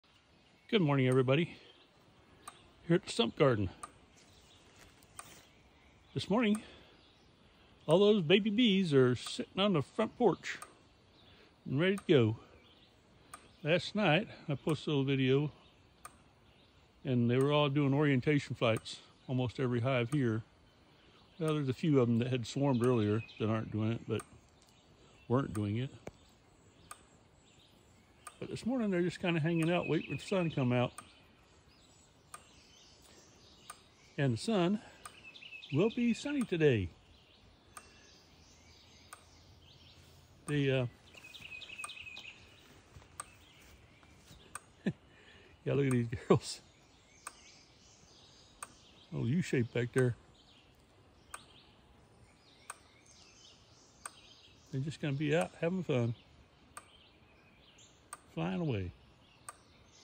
Lady Bees Start Your Engines!
Thousands of nurse bees emerged this past rainy week forcing older nurse bees to become foragers. Now, the sun has come out for a change and after the rain stopped last afternoon they emerged from the beehive. When they came back there was no room at the inn. Now they are waiting to take flight again!